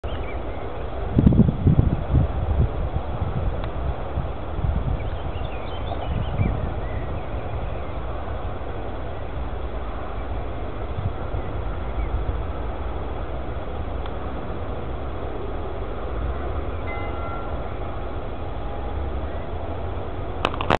IL MINIFLOWFORM
Variante del Flowform ma con profilo personale dimensioni 50 x70 centimetri !!